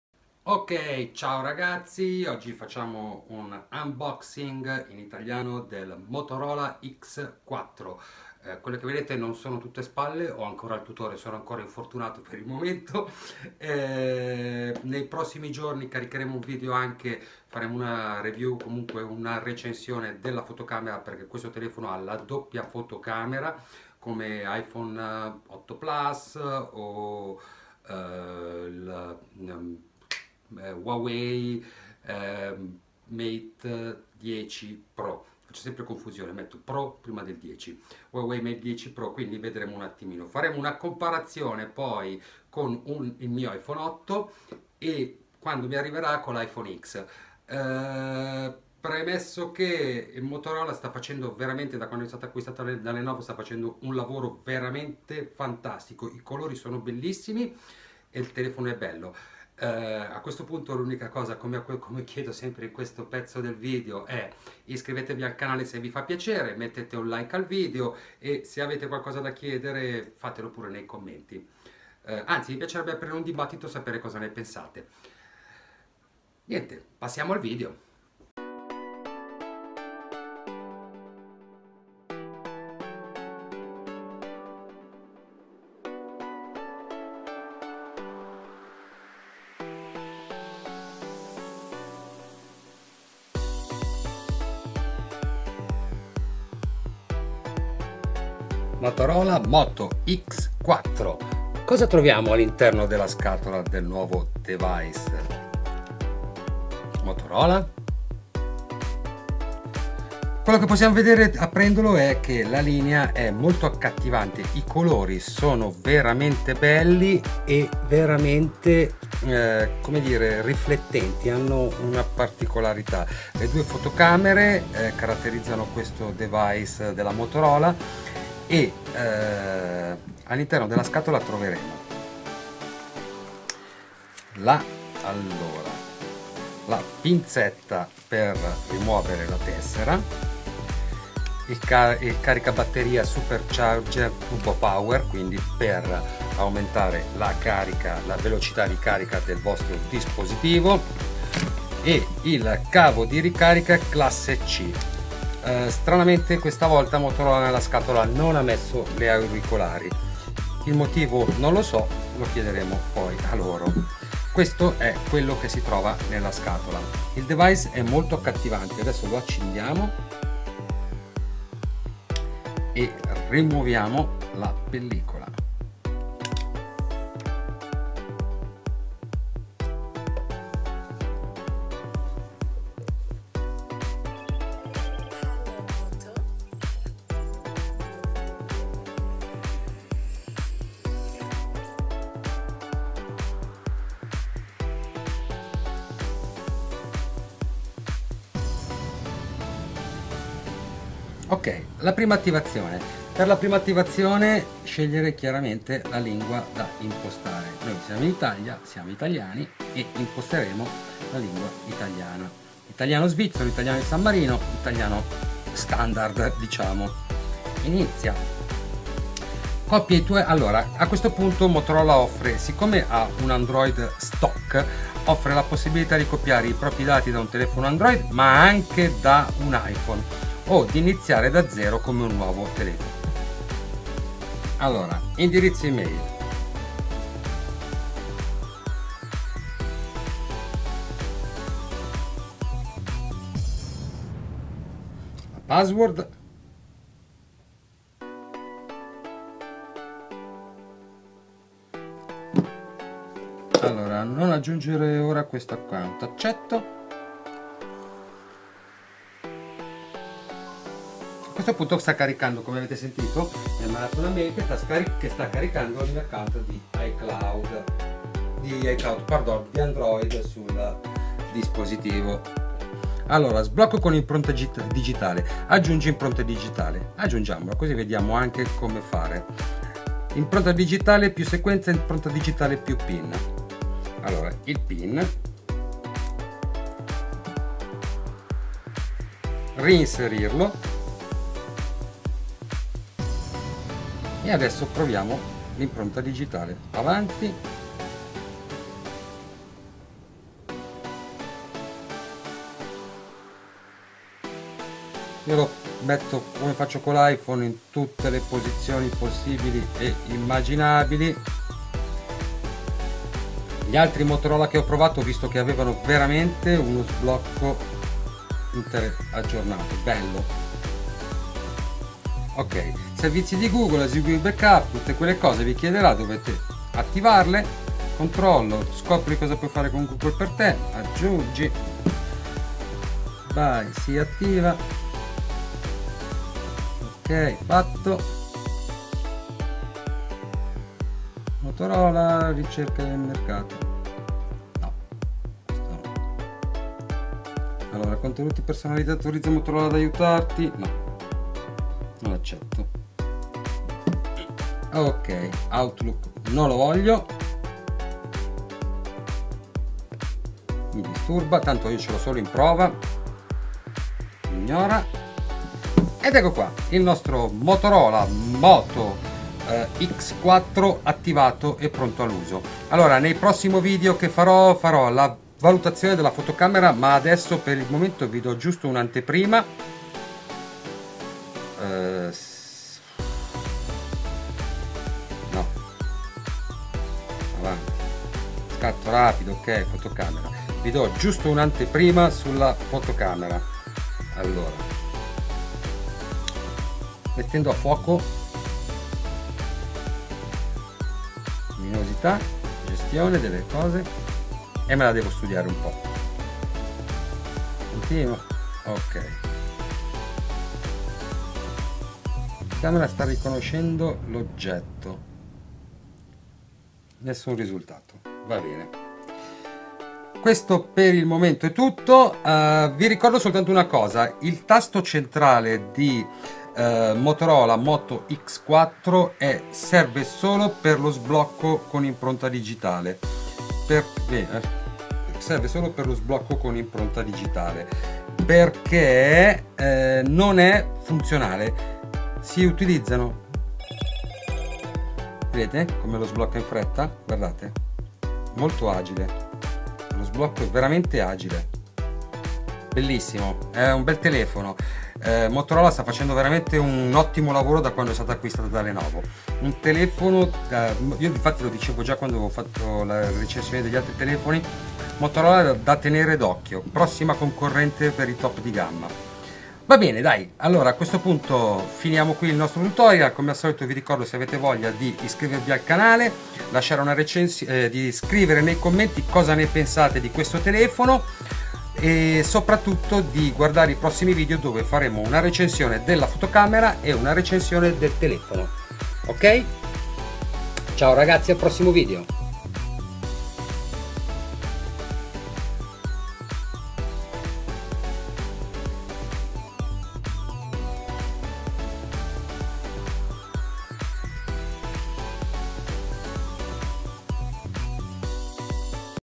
Motorola MOTO X4 UnBoxing italiano e prime impressioni e attivazione con Android one
Motorola Moto X4 unboxing in italiano, con prima attivazione e le prime impressioni del nuovo fascia media di Motorola con Android one la stock no custom quindi pura.

Ho avuto in anteprima questo dispositivo da vedere e fare una recensione.

Nei prossimi video approfondirò le mie impressioni sulla fotocamera.

iMania assistenza Varese